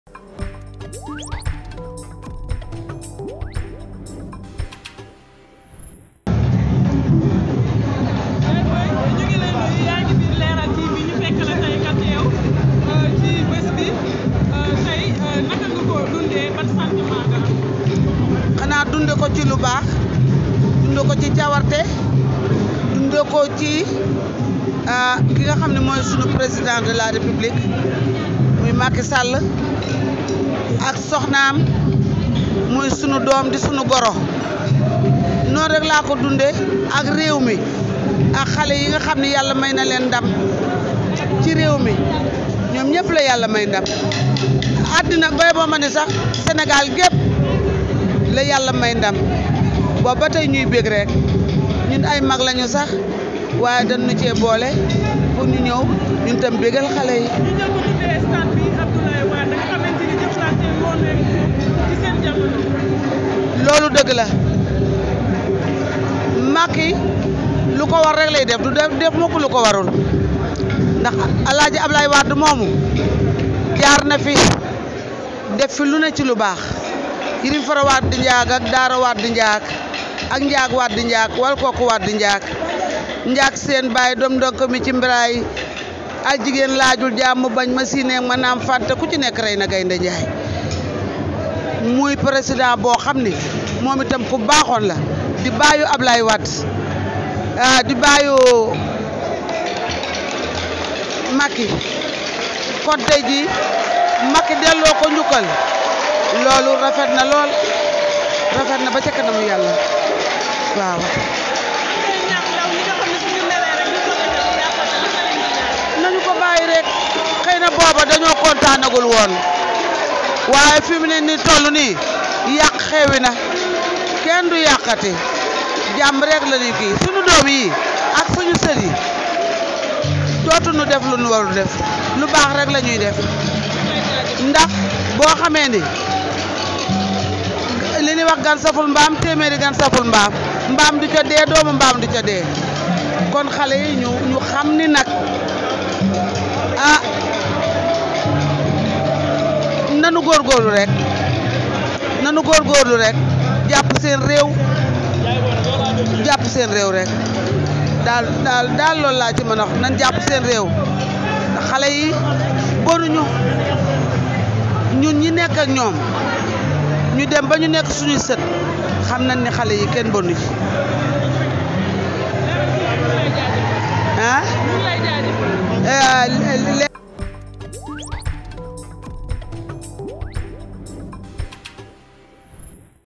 Daro Mbaye: "Cette jeunesse est consciente de ce que Macky Sall a fait pour elle avec ce bijou"
Dara Mbaye fait figure d'ancienne gloire de la chanson. A l'en croire, Macky Sall ne fait que ce qu'il doit faire. "Il a honoré tout le Sénégal en honorant Abdoulaye Wade", dit-elle. Elle est sûre que la jeunesse gardera jalousement ce joyau en lieu et place des casses qu'elle a l'habitude de faire. "Elle est consciente de ce que Macky Sall a fait pour elle"